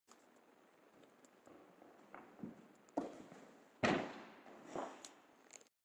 Letní soustředění 2013 - Noemi - přeskok - průpravy na Jurčenko
Noemi trénuje gymnastiku teprve půl roku. Zatím jí chybí základy, ale když holky něco trénují, zkouší se přidat a leccos jí už jde skoro stejně jako jim.